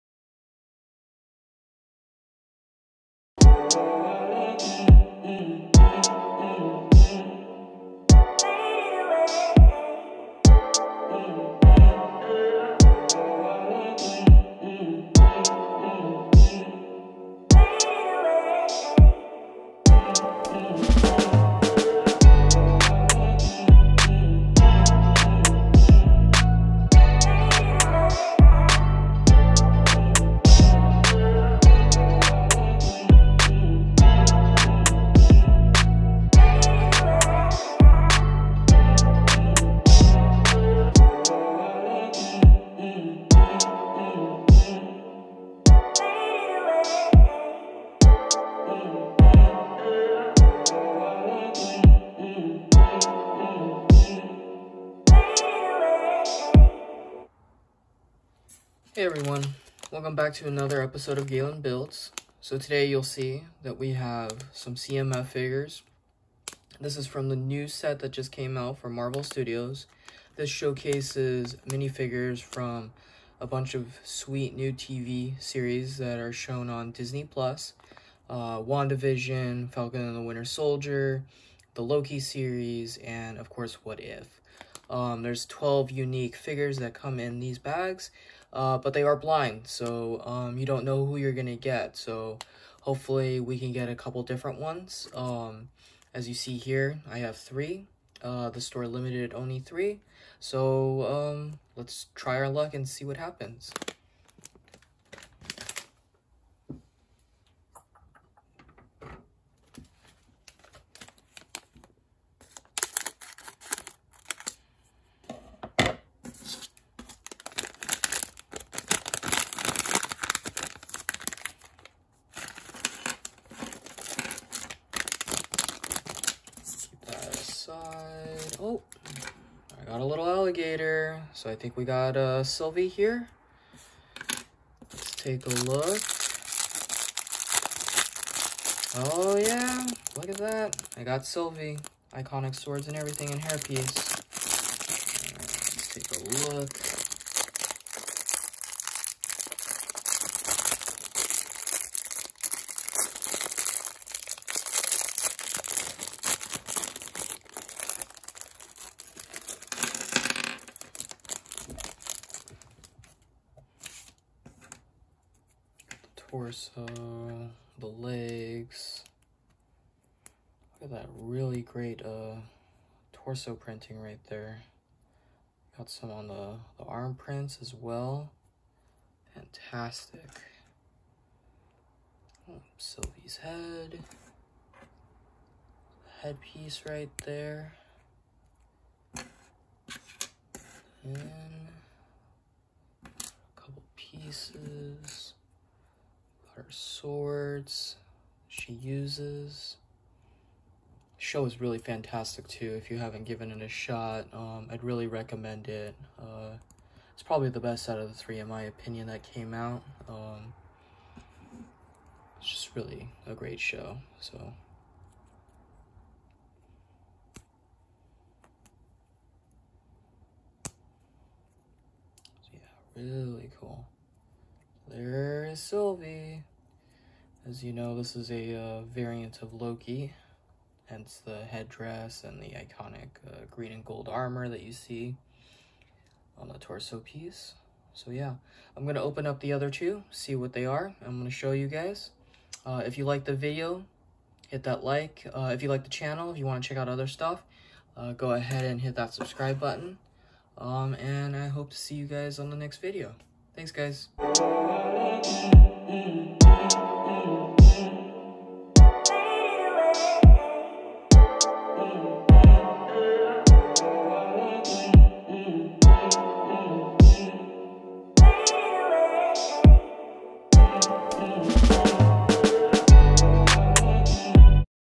Lego Store Visit and Haul!
So as I continue to build the channel and showcase awesome sets that I both build and talk about, I also want to show my visits and hauls for future videos. This one shows my time at the Lego Store at Southcenter Mall located in Tukwila, Washington. If you get the chance to visit the location, do so, the staff are all helpful, knowledgeable and the stock is always fantastic!

Thanks and see you at the next build!